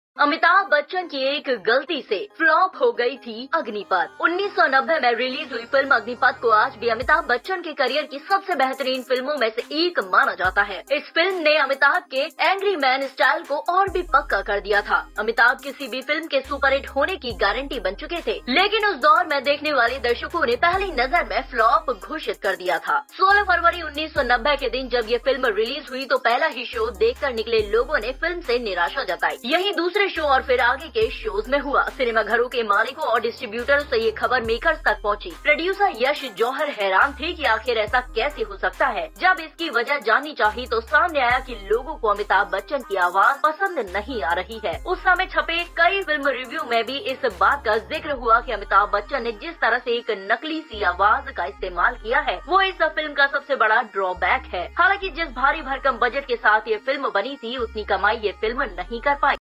अमिताभ बच्‍चन की एक गलती से फ्लॉप हो थी अग्‍न‍िपथ
The film 'Agneepath' released in 1990 is still considered one of the best films of Amitabh Bachchan's career. This film further confirmed Amitabh's 'angryman' style. Amitabh had become a guarantee of any film being a super hit. But the audience watching at that time called 'Flop' at first sight.
had been declared. When this film was released on February 16, 1990, the people who came out after seeing the first show expressed their disappointment with the film. This happened in the second show and then in subsequent shows.

This news reached the makers from theater owners and distributors. Producer Yash Johar was surprised as to how this could happen. When wanted to know the reason for this, it came to light that people are not liking Amitabh Bachchan's voice. It was also mentioned in many film reviews published at that time that the way Amitabh Bachchan has used a fake voice, it is the biggest draw-back of this film. However, with the huge budget with which this film was made, this film could not earn that much.